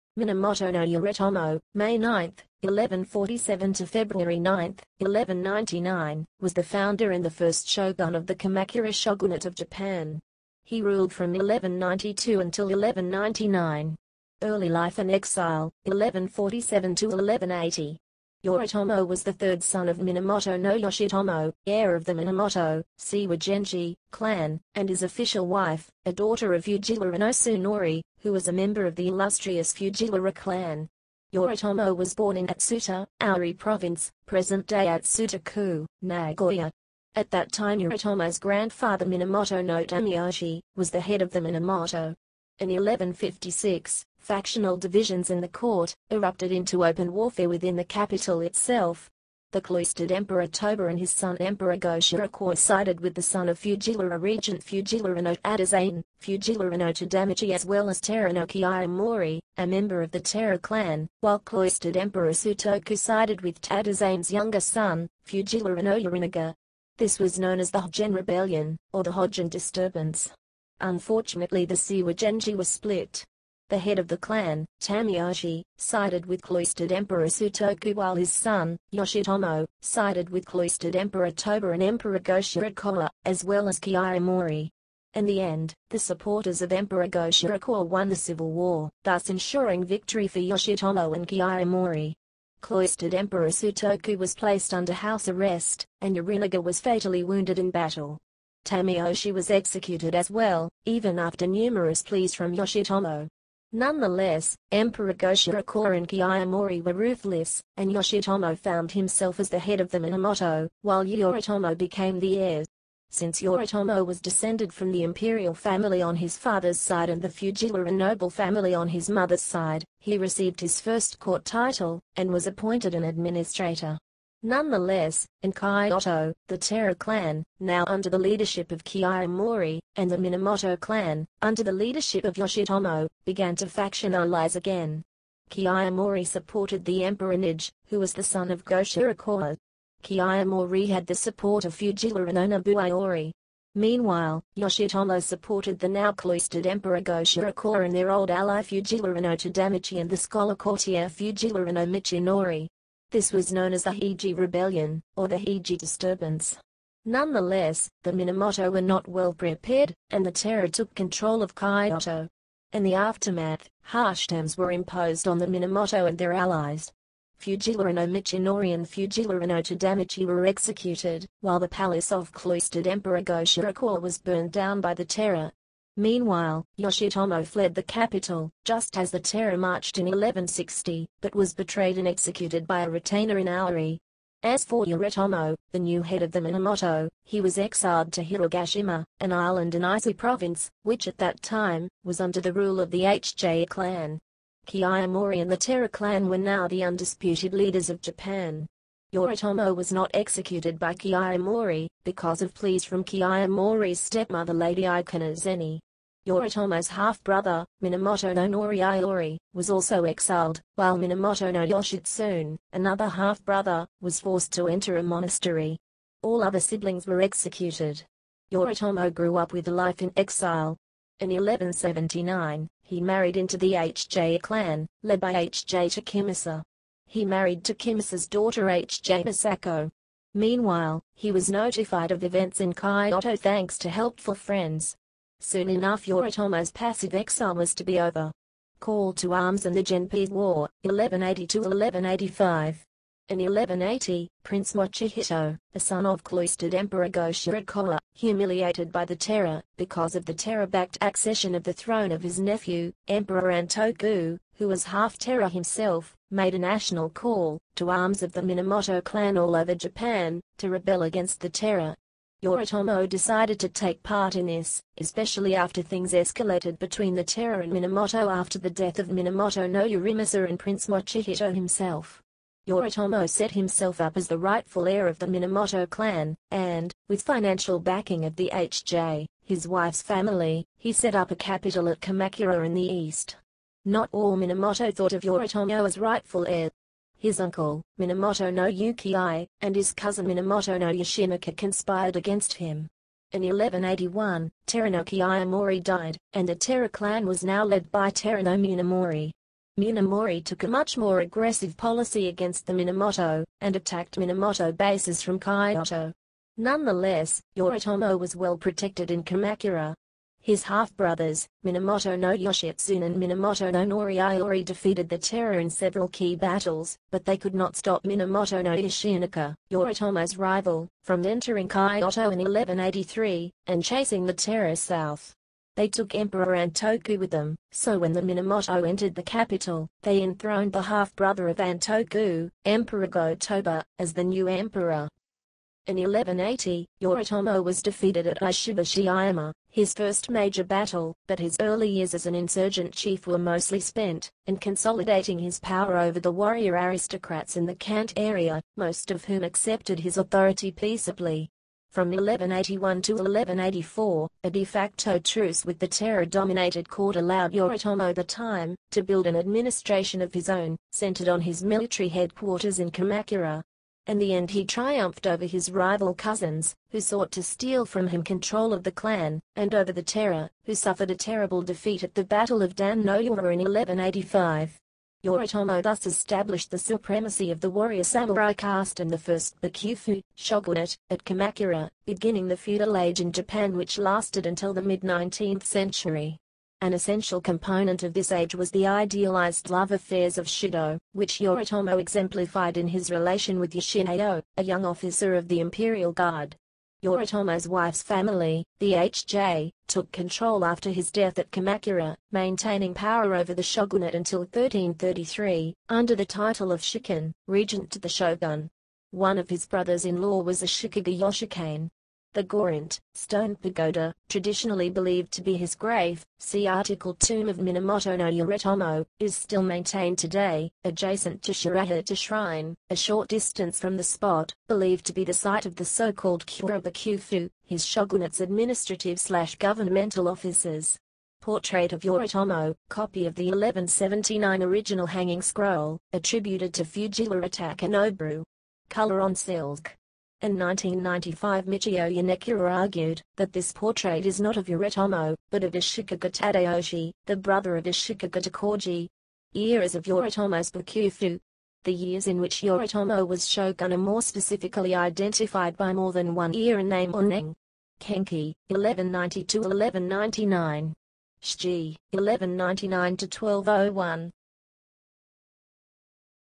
Minamoto no Yoritomo
Yoritomo was the third son of Minamoto no Yoshitomo, heir of the Minamoto (Seiwa Genji) clan, and his official wife, a daughter of Fujiwara no Suenori, who was a member of the illustrious Fujiwara clan. Yoritomo was born in Atsuta, Owari Province(present-day Atsuta-ku, Nagoya). At that time Yoritomo's grandfather Minamoto no Tameyoshi, was the head of the Minamoto.